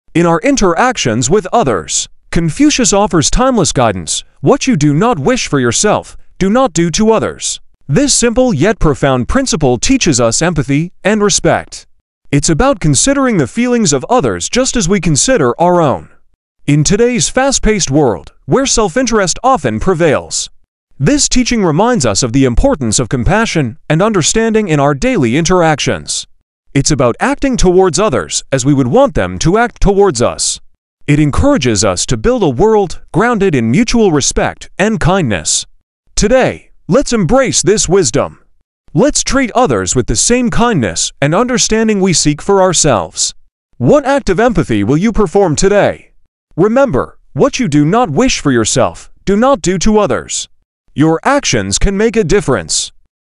Ancient Voices, Modern Echoes : Confucius "What you do not wish for yourself, do not do to others."
Description:
In this enlightening episode, we dive deep into one of Confucius' most profound and enduring teachings: "What you do not wish for yourself, do not do to others." This simple yet powerful statement, often compared to the Golden Rule, has echoed through the ages, influencing millions across generations and cultures.